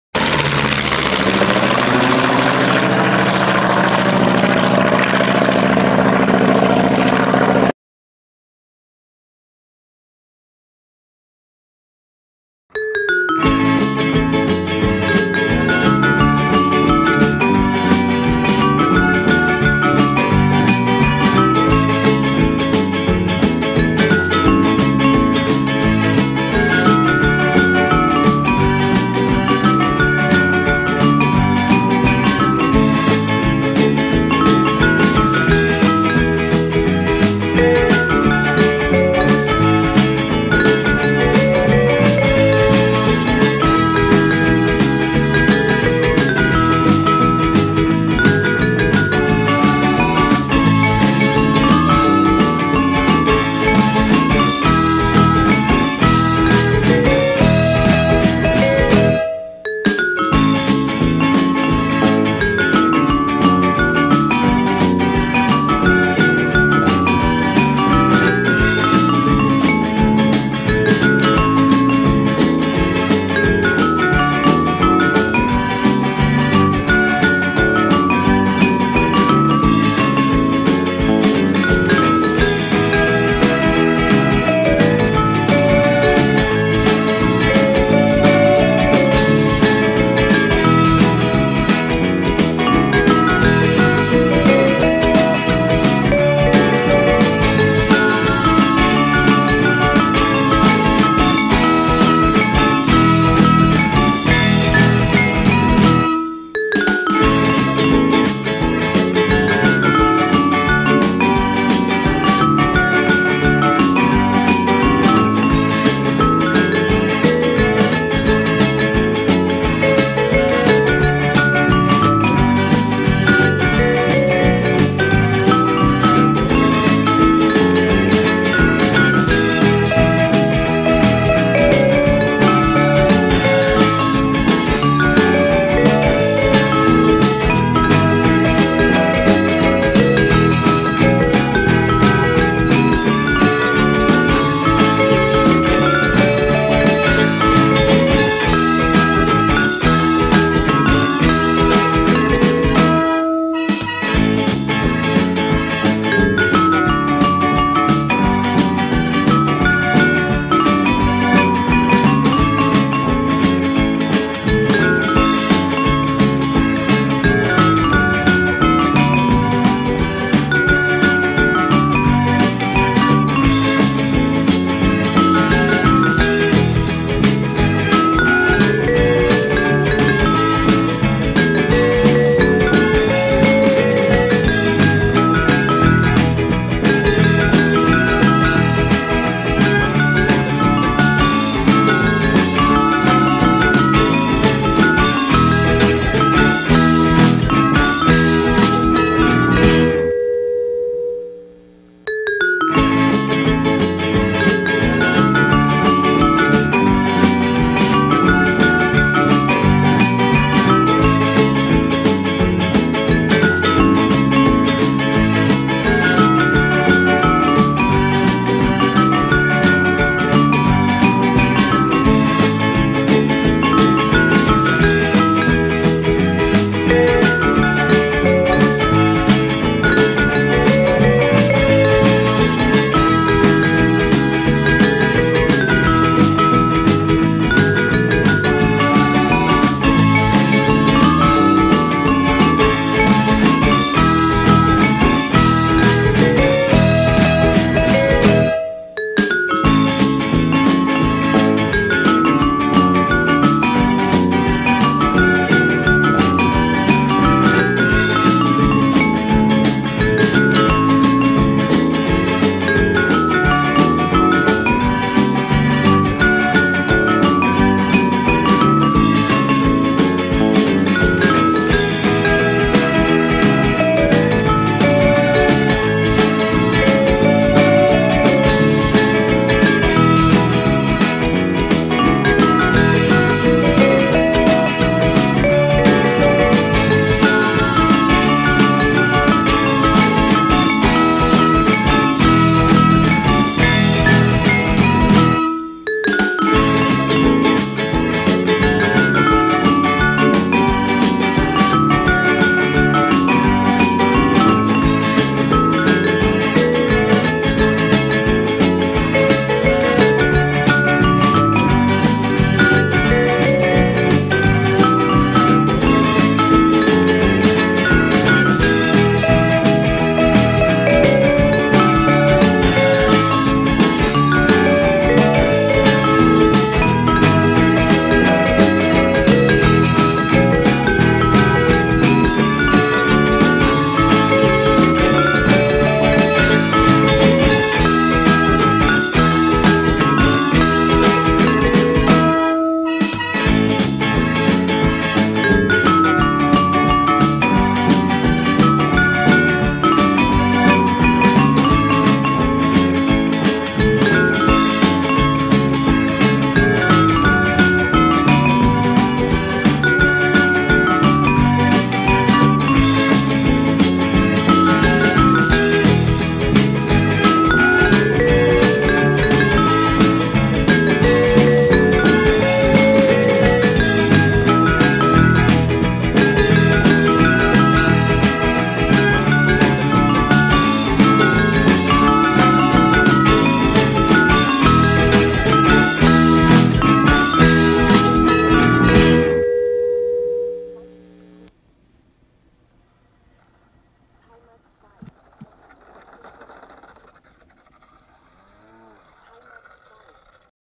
Klemm 25 D Frühlingserwachen
Die Klemm zeigt ein paar Kunstflugeinlagen bei herrlichem Frühlingswetter.